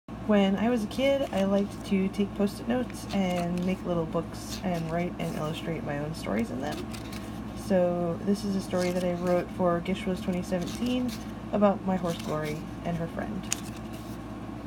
GISHWHES 2017 - Item 2 - Hobby Revisited
The story reads
The Two Mares By Liz Staley
Once there were two mares named Glory and Liberty.
They lived in a pasture and ate hay together all day.
Liberty was a horse and Glory was a pony.
Glory loved apples but Liberty didn't care for them.
Glory loved to pull and cart and Liberty liked to jump!
They were very different from each other-
They were even different breeds of horse.
But they played together in the summer sun.
And they enjoyed the shade of the trees together.
Even though they were very different, they were the best of friends!